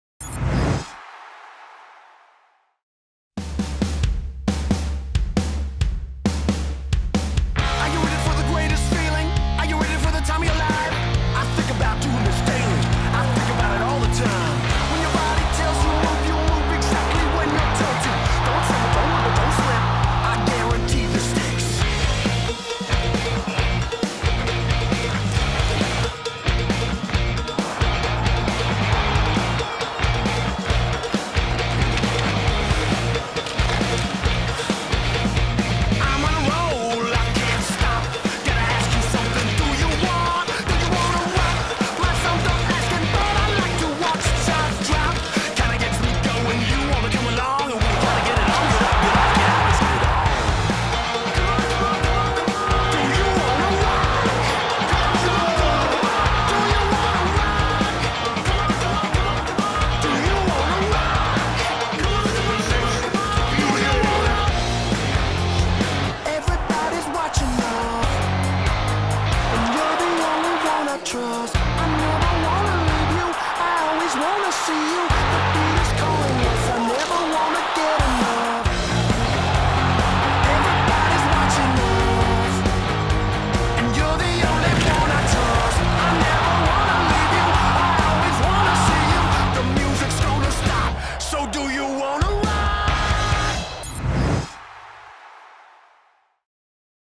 GamePlus™ Stanley Cup Playoffs Mashup: Senators at Canadiens
Experience the NHL in a whole new way with Rogers Game Plus™. Watch the best angles from Game 1 between the Ottawa Senators and Montreal Canadiens.